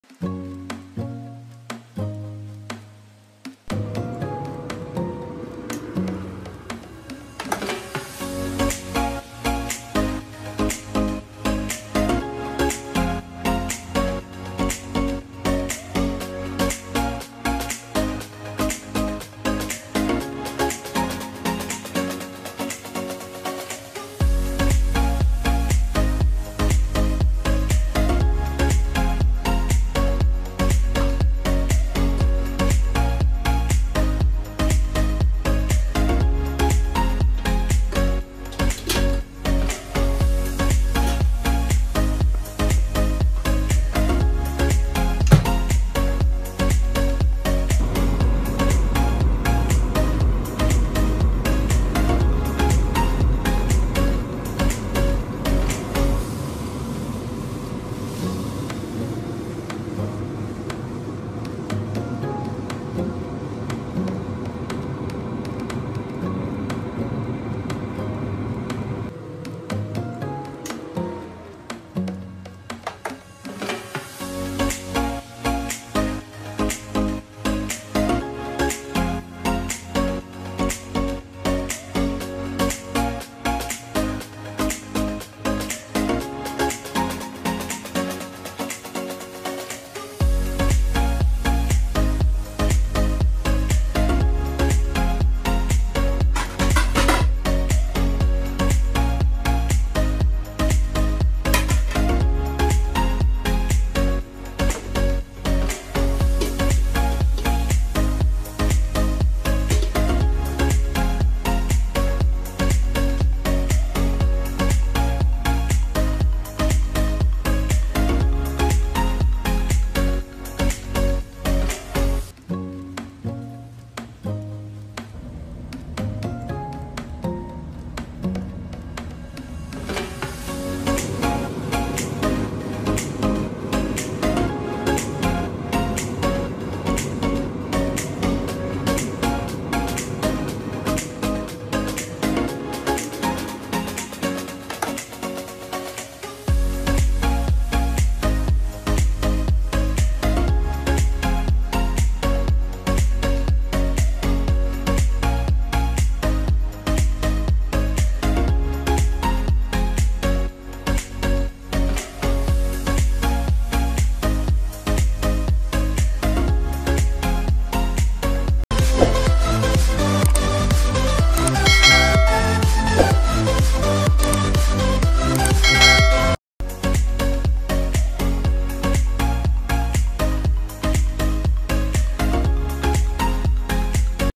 Air Fryer | Banana Cue | Jhaspatula
Healthier way of cooking.

Banana Cue

Ingredients:
Banana (Saging na Saba)
Brown Sugar